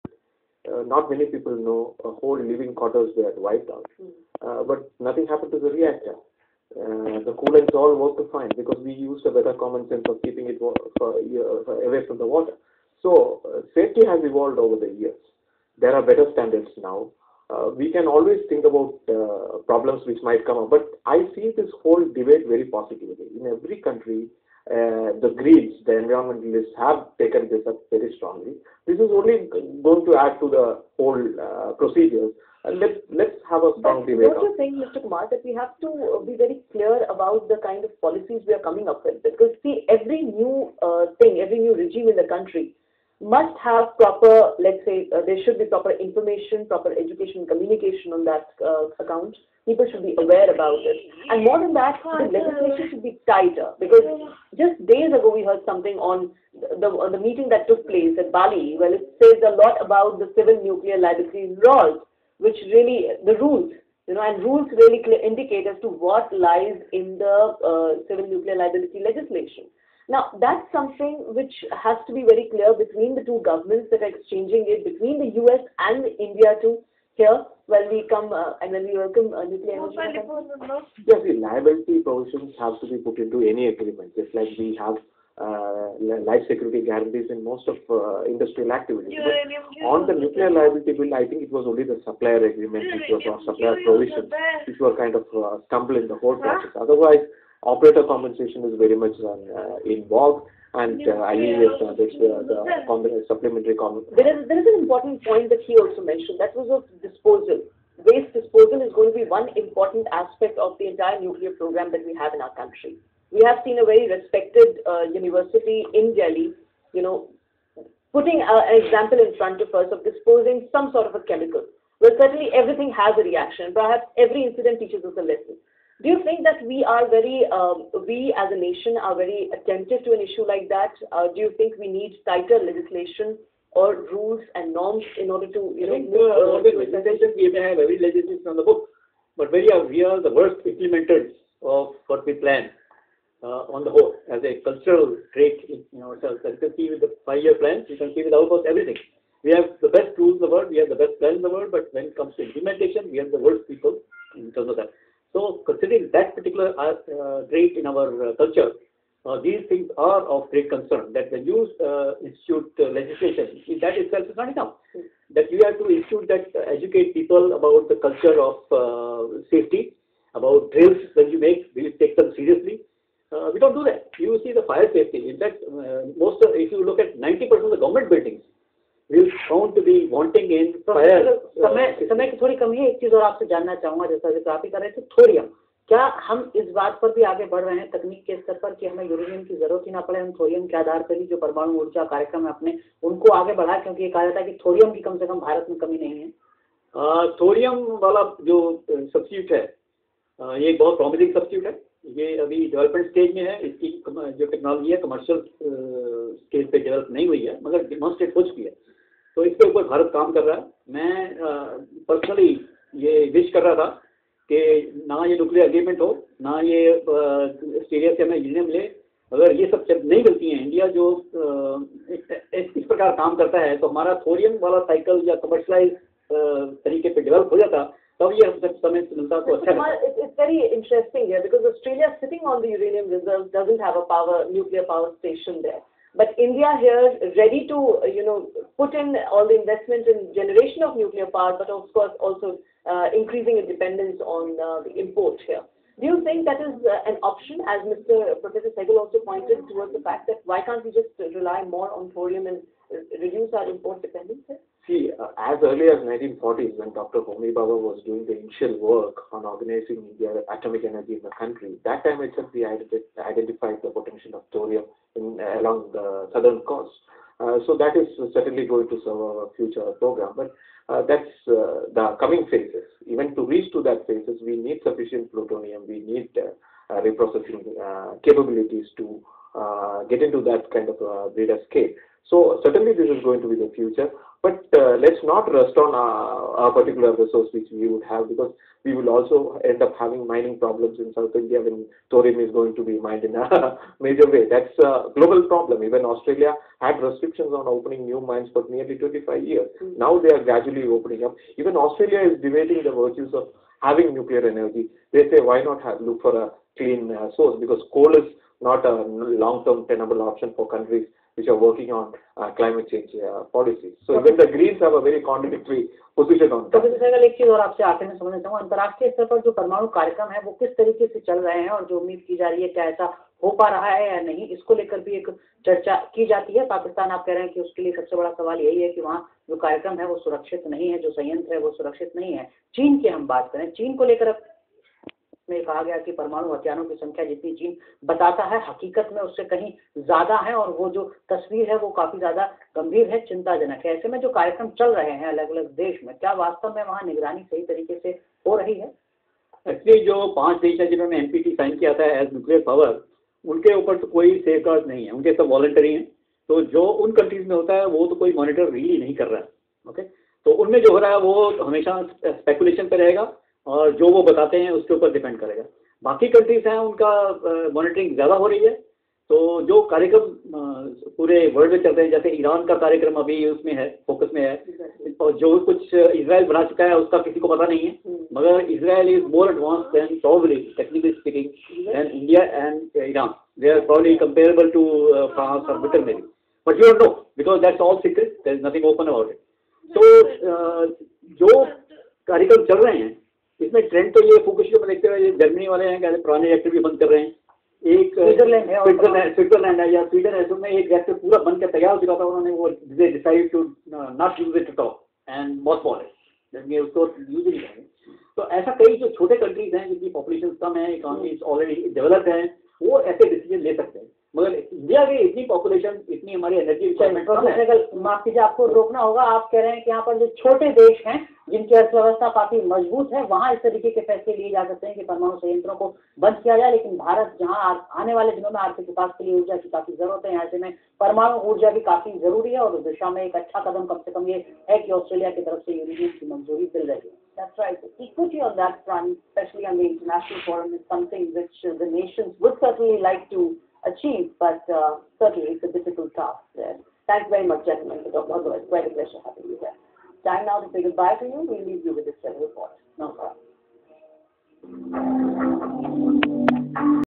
Discussion on Australian Uranium to India in Lok Sabha TV